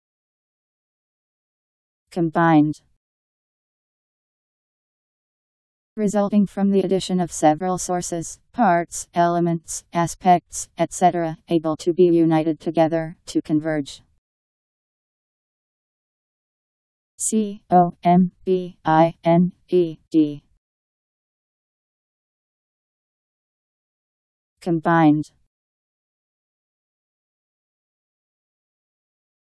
Combined Meaning
Video shows what combined means. Resulting from the addition of several sources, parts, elements, aspects, etc. able to be united together, to converge..  Combined Meaning. How to pronounce, definition audio dictionary. How to say combined. Powered by MaryTTS, Wiktionary